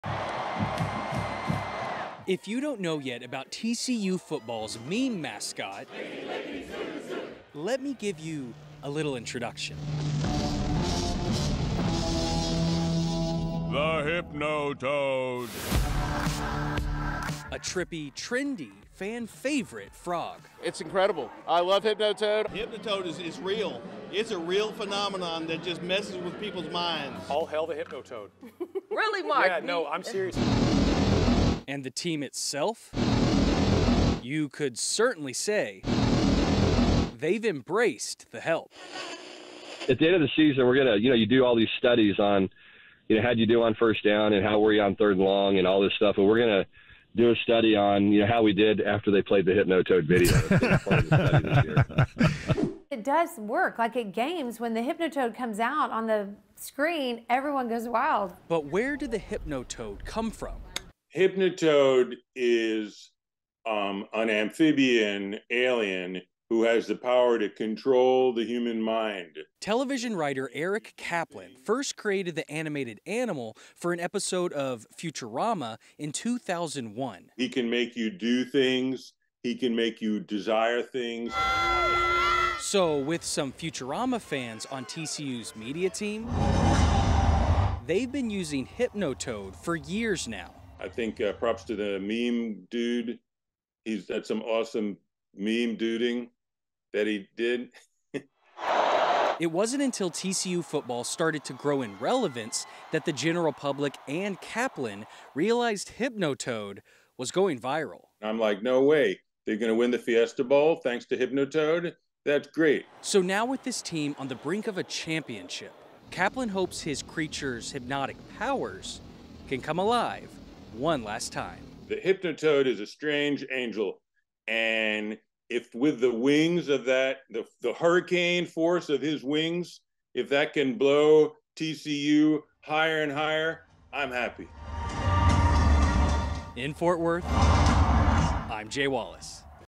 Hypnotoad creator says it's 'awesome' TCU players and fans embrace his creation
Whether in Amon G. Carter Stadium, in downtown Fort Worth or scrolling through social media on a Saturday while watching college football, it's nearly impossible to avoid interacting with TCU's viral new, meme mascot.

The Hypnotoad.

The four-legged, mesmerizing amphibian has become a rallying symbol of TCU's undefeated season so far.

For the uninitiated, the Hypnotoad is from Matt Groening's futuristic animated show, "Futurama."

"The Hypnotoad is a large toad-like creature that has large oscillating multicolored eyes, and emits a droning hum. His eyes glow all the time. The toad apparently uses these as a power of hypnotism on surrounding people and animals."

But we at WFAA wanted to go a step further than simply relying on a fan's website. We wanted to go to the source — the source of Hypnotoad.

That's where television writer Eric Kaplan comes into the picture. The New York native started his career with "Late Show with David Letterman" and eventually joined "Futurama" as one of the writers.